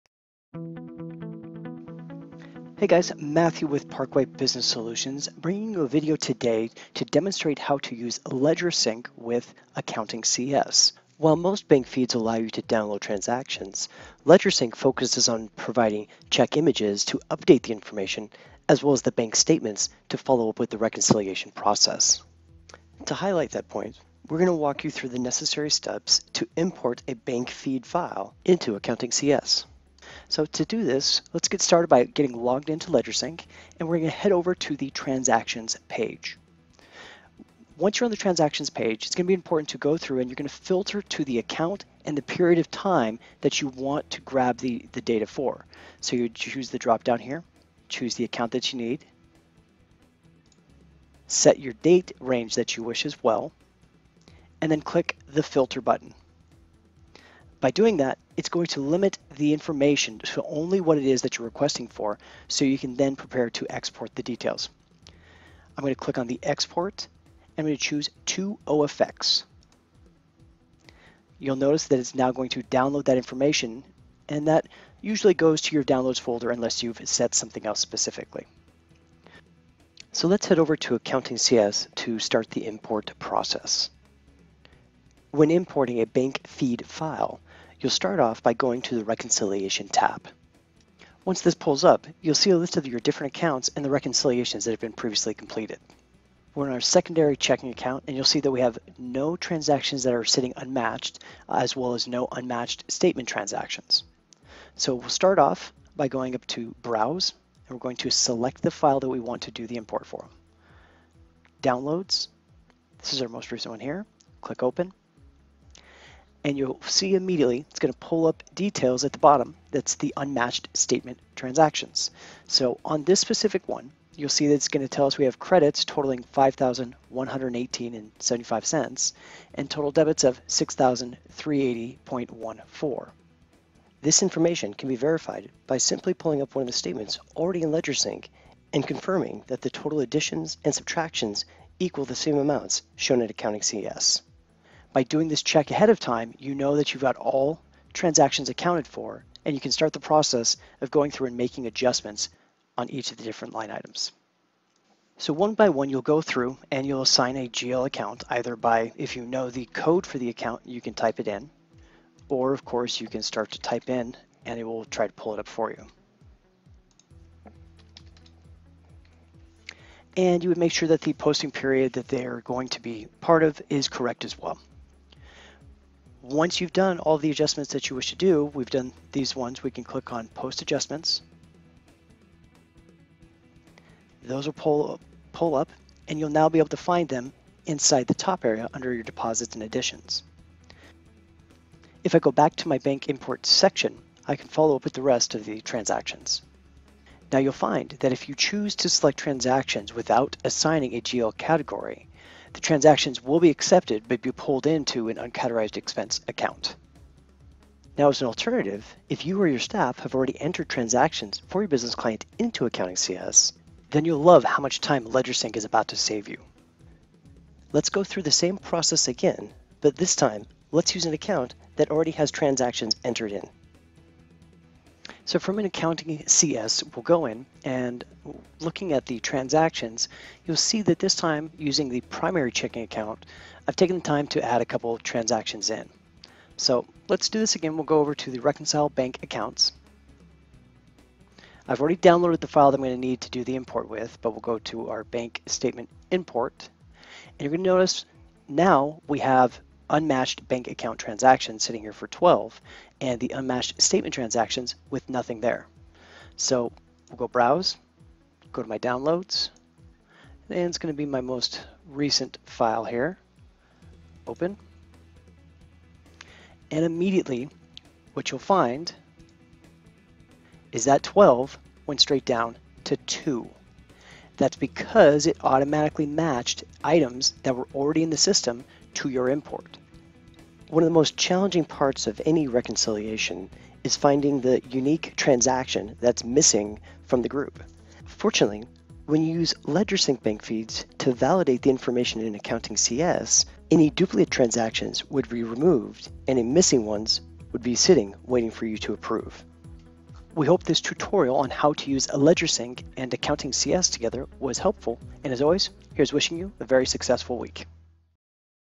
Reduce your data entry with Ledgersync and Accounting CS
How to save time entering transactions into Accounting CS with Ledgersync.  Quickly and easily view all your check images,  and bank and credit card statements from a secure portal.